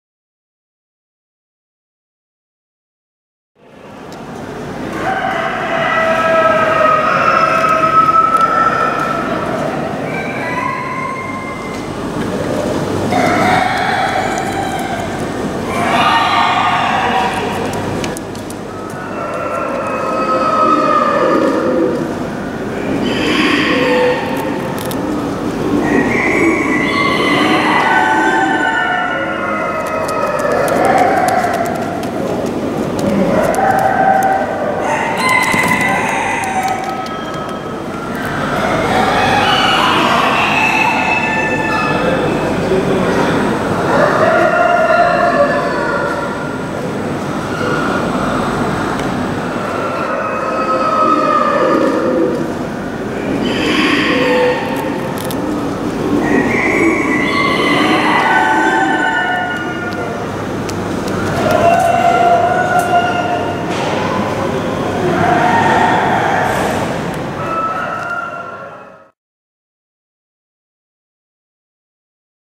Pigeon Boulant Horseman - Cropper - Pouter - Kröpfer - Exposition Avicole d'Évreux
Mâle et femelle adultes bleus barrés à Bruno BOUILLET - Salon de l'Aviculture d'Évreux 2019 - Organisation S.A.E. ( Société d'Aviculture de l'Eure ).